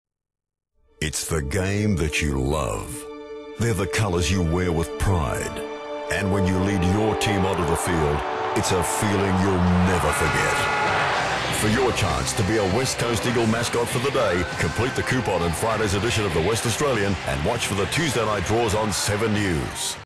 West Coast's Mascot for a Day
Young West Coast fans have the chance to lead their team out to a roaring home crowd at Patersons Stadium